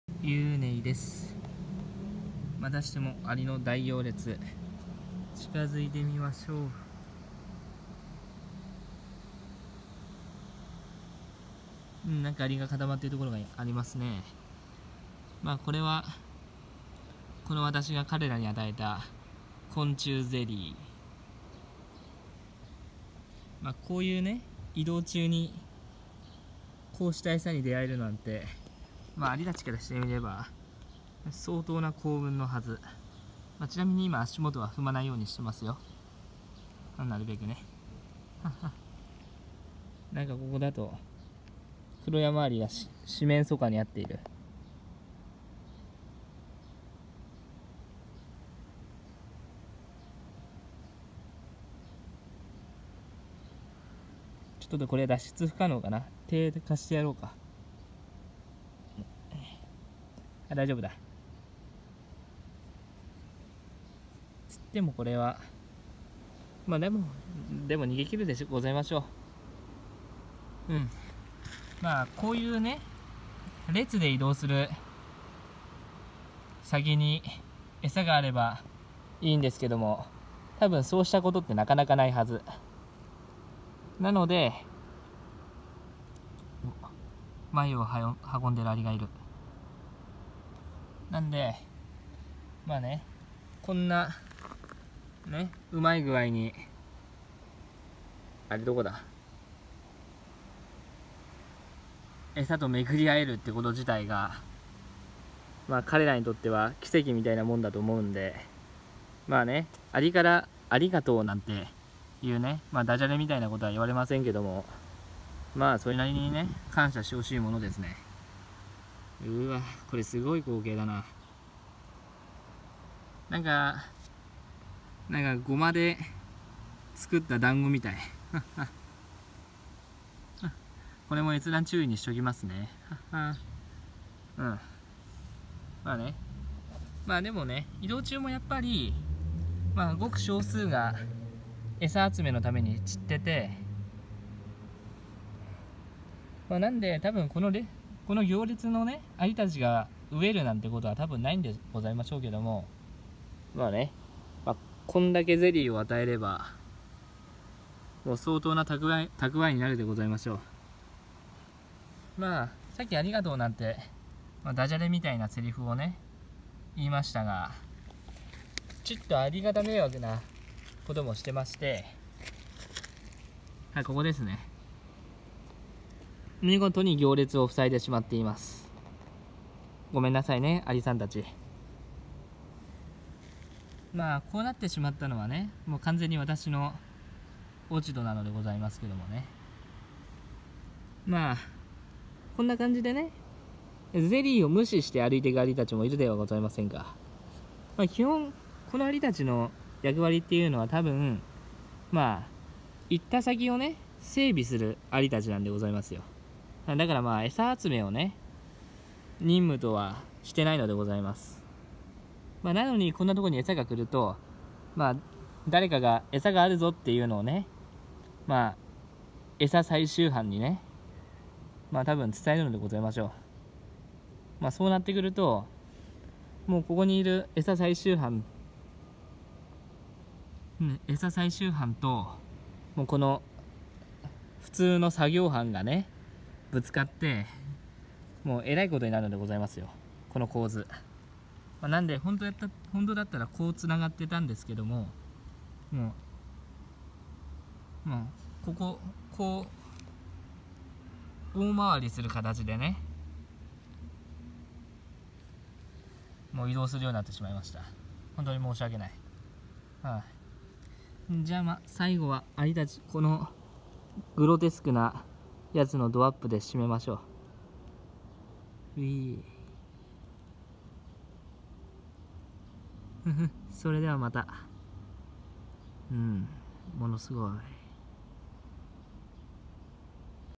アリの行列再び 昆虫ゼリーを与えたらどうなる？
どうも、ユウネイです。今回もたまたまアリたちの行列を発見しました。

昆虫ゼリーを行列のアリたちに与えたところ、ごま団子🍡みたいなアリの塊ができてしまいました。行列がこんなごちそうにありつけることなんてそうそうないはず、また昆虫ゼリーは自然界では貴重なタンパク質と糖分などを含んでいるので、がっつきようが凄まじかったですね。しかしそこは役割分担が得意なアリ、ちゃんと歩くアリは歩いて、餌を取るアリは取っていました。アリの群れは本当によくできてます。